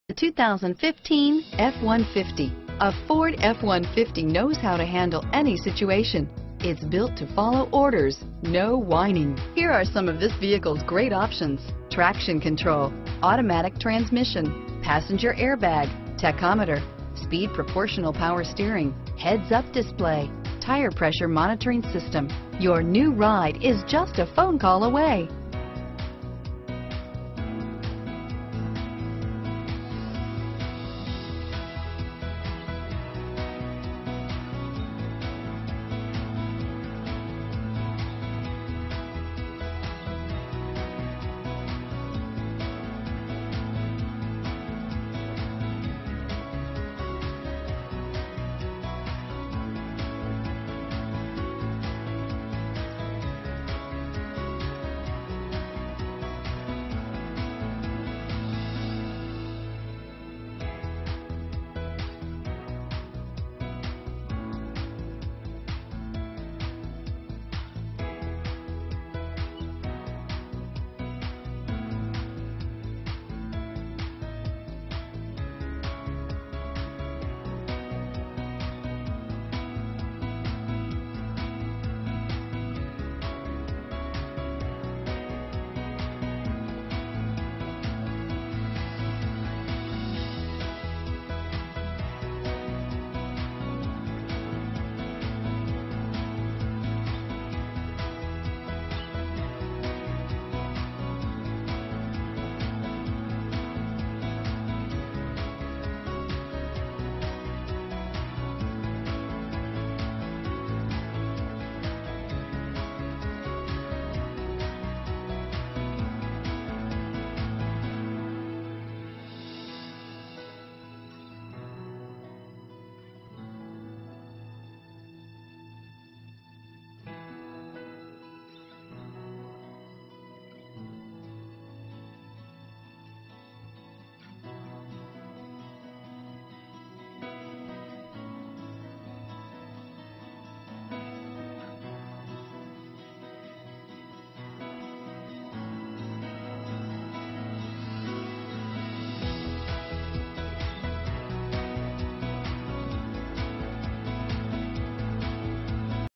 2015 Ford F-150 Centennial CO, Littleton CO, Fort Collins CO, Greeley CO, Cheyenne WY FKE86337
2015 Ford F-150
2015 Ford F-150 4WD SUPERCAB - Stock#: FKE86337 - VIN#: 1FTFX1EFXFKE86337

O'Meara Ford
400 W. 104th Ave.

Stunning!!! New In Stock*** Gassss saverrrr!!! 21 MPG Hwy.. This considerable F-150, with its grippy 4WD, will handle anything mother nature decides to throw at you! Get down the road in this hardy Vehicle, and fall in love with driving all over again.. Safety equipment includes: ABS, Traction control, Curtain airbags, Passenger Airbag, Stability control - Stability control with anti-roll...Other features include: Auto, Tilt and telescopic steering wheel, 4 Doors, 4x4, Fuel economy EPA highway (mpg): 21 and EPA city (mpg): 15...